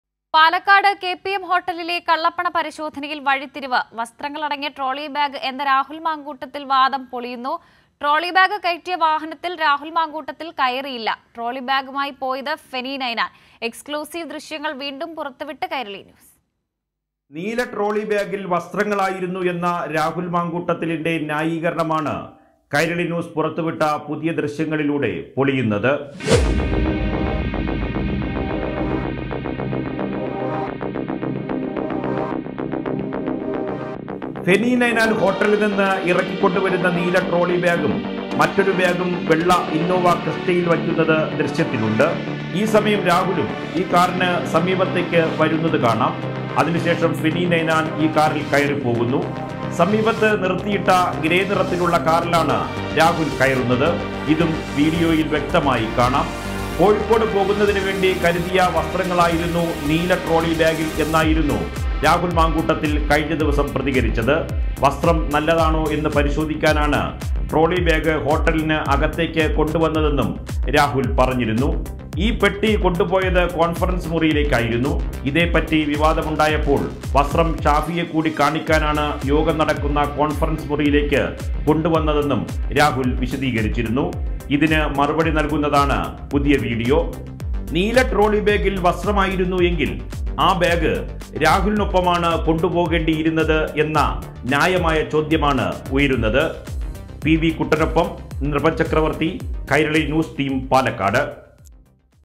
KPM ഹോട്ടലിൽ നിന്ന് രാഹുൽ പുറത്തുപോയത് മറ്റൊരു വാഹനത്തിൽ;CCTV ദൃശ്യങ്ങൾ കൈരളി ന്യൂസിന് | Congress
കെപിഎം ഹോട്ടലിൽ നിന്ന് രാഹുൽ പുറത്തുപോയത് മറ്റൊരു വാഹനത്തിൽ; കൂടുതൽ സിസിടിവി ദൃശ്യങ്ങൾ കൈരളി ന്യൂസിന്
Black money in palakkad; Rahul Mamkootathil 's arguments fall apart